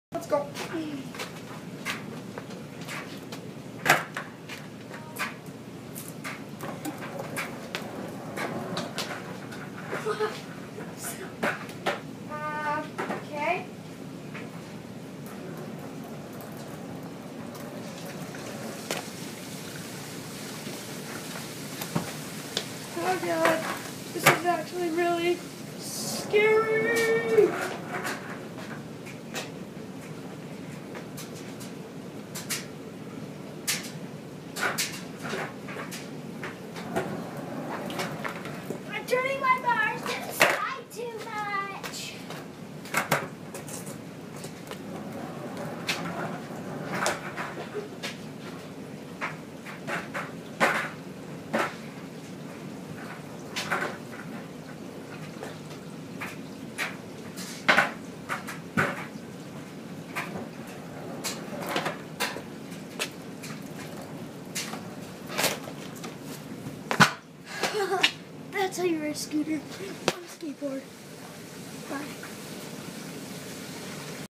scootering on a SKATE BOARD fun
aaaaaaa i almost fell on scrap wood :0